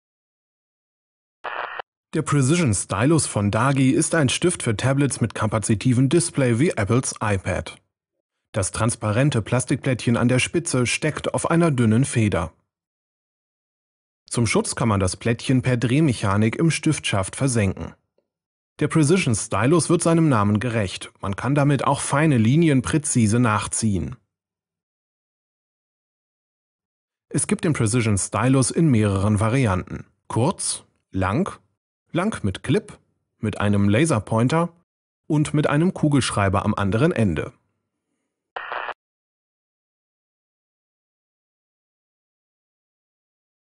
Dagi Precision Stylus -- besserer Tablet-Stift
- Wer mit einem Stift auf einem Tablet schreiben oder zeichnen will, wird die Stifte des taiwanischen Herstellers Dagi mit dem transparenten Plättchen an der Spitze kennen. Bei den neuen Modellen kann man dies nun beim Transport einziehen.